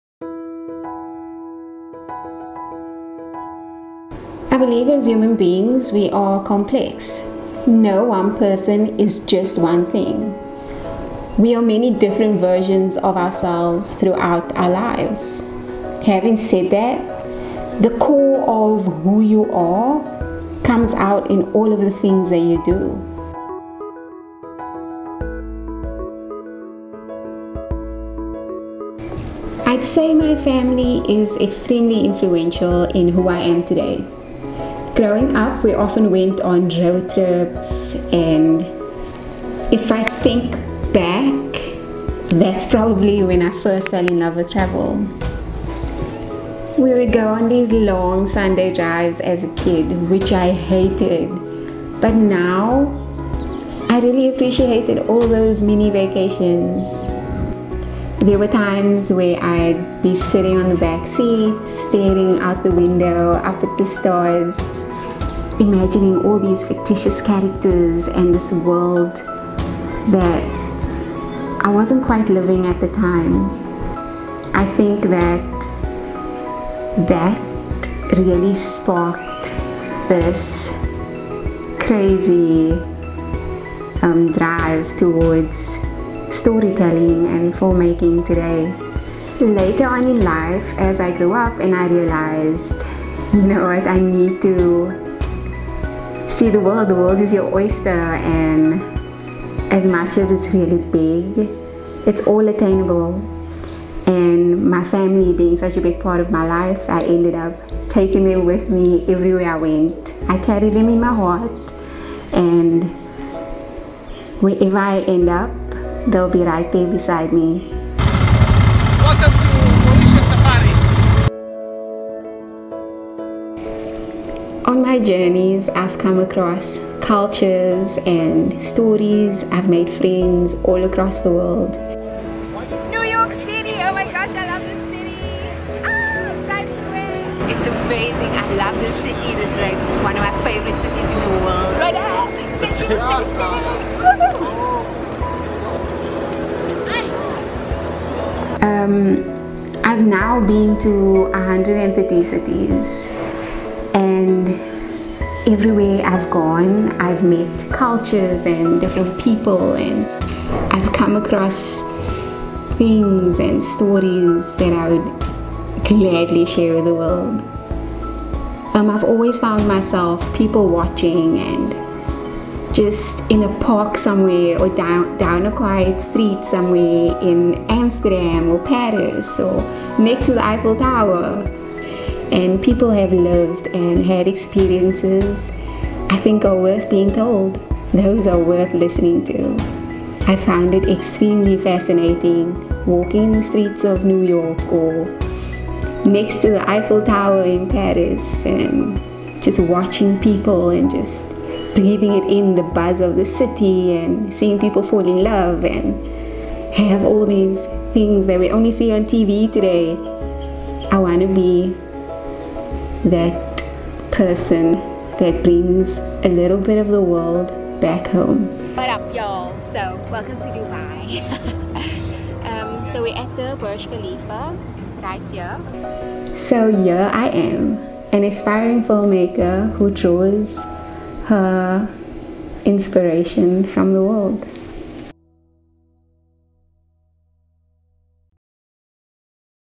This is me | A Self Portrait | Living vicariously through myself!
A filmmakers self portrait is basically just an introduction to who you are and where  your interests are stemming from. I fell in love with film because story interests me. Imagining a fictitious world allows me the freedom to create images in my mind that made me laugh, smile or even cry. Filmmaking to me is bringing my dreams to life. It is an escape from a dull and ordinary existence.

I hope this self portrait helps you create one of your own, and because we grow and constantly change i am sure the next one i do, will reflect some of that in my life.

Timestamps
00:00 Intro
00:04 Who we are as human beings
00:28 Who influences who i am
00:47 What influences played a role
01:15 What sparked the love of film
01:40 How travel started
03:47 Who i am today

I do appreciate the love and support if you liked it sub it!